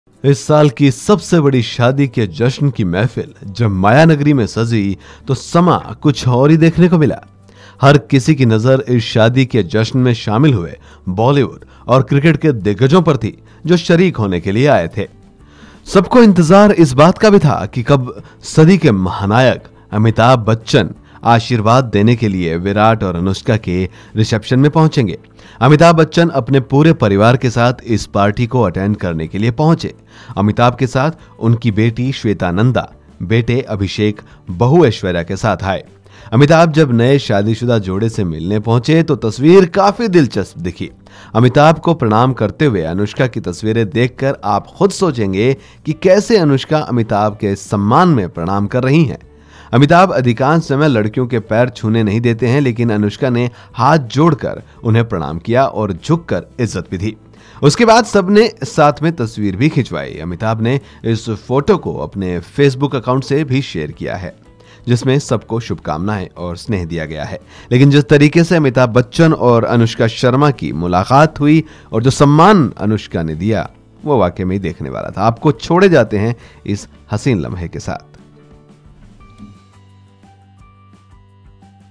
Virat Kohli Anushka Sharma ने कुछ यूँ किया Amitabh Bachchan का सम्मान | Boldsky
Amitabh Bachchan made it a point to attend Anushka Sharma and Virat Kohli's wedding reception in Mumbai, despite having his arm in a sling. Daughter Shweta was present to lend a supporting hand, while Abhishek and Aishwarya followed close behind.

इस साल की सबसे बड़ी शादी की जश्न का महफिल जब मायानगरी में सजा तो समा ही कुछ और देखने को मिला.. हर किसी की नजर इस शादी के जश्न में शामिल हुए बॉलीवुड और क्रिकेट के दिग्गजों पर थी जो शरीक होने के लिए आए थे... सबको इतंजार इस शादी का था की कब सदी के महानायक आशीर्वाद देने के लिए विराट और अनुष्का के रिशेप्शन में पहुंचेगे... अमिताभ अपने पूरे परिवार के साथ इस पार्टी को अटेंड करने के लिए पहुंचे... अमिताभ के साथ उनकी बेटे श्वेता नंदा, बेटे अभिषेक बहु ऐश्वर्या साथ थे... अमिताभ जब नए शादी शुदा जोड़े से मिलने के लिए पहुंचे तो तस्वीर काफी दिलचस्प दिखी.. अमिताभ को प्रणाम करते हुए अनुष्का की तस्वीर देख आप खुद सोचेंगे की कैसे अनुष्का अमिताभ के सम्मान में प्रणाम कर रही है... अमिताभ अधिकांश समय लड़कियों को पैर छुने नहीं देते इसलिए अनुष्का ने हाथ जोड़ कर  उन्हें प्रणाम किया और झुक कर इज्जत भी दी... उसके बाद सबने साथ में तस्वीर भी खिंचवाई... अमिताभ ने इस फोटो को अपने फेसबुक एकाउंट से भी शेयर किया है... जिसमें सबको शुभकामनाएं और स्नेह दी है...